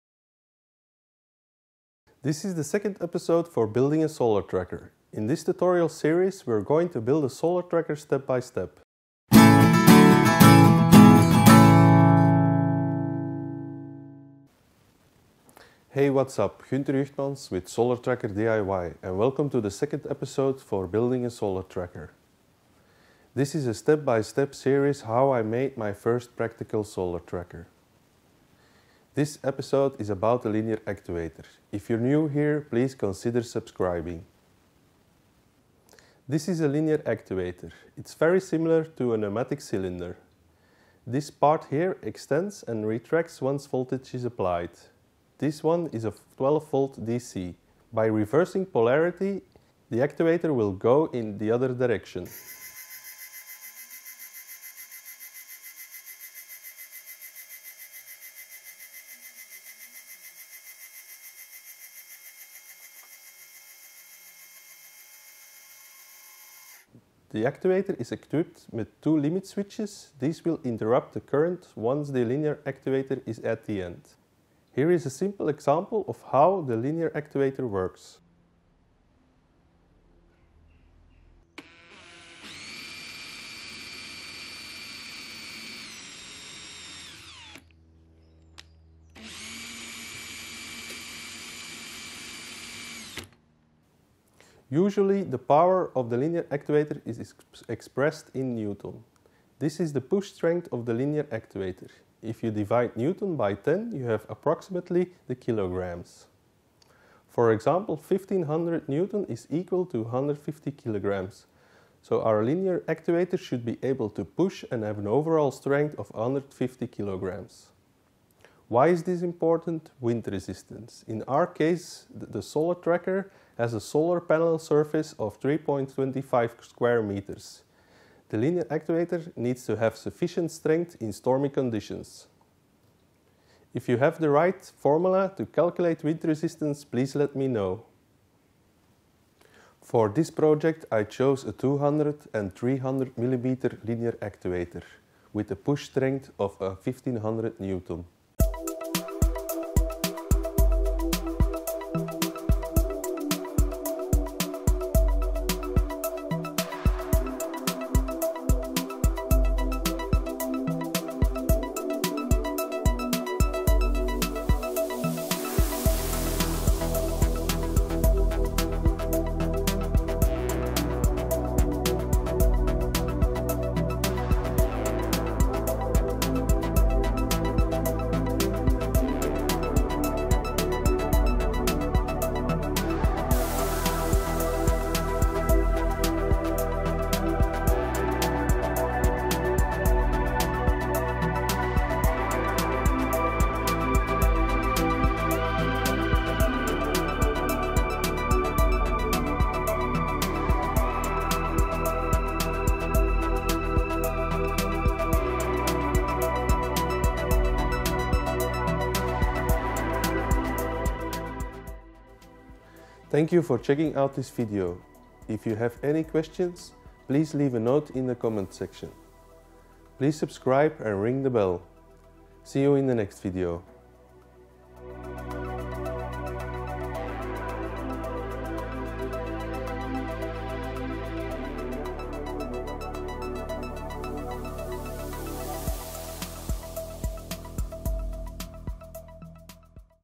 Tutorial 02 Solar Tracker: The Linear Actuator
This is the second episodes for building a solar tracker.
In this episode we take a closer look at the Linear actuator.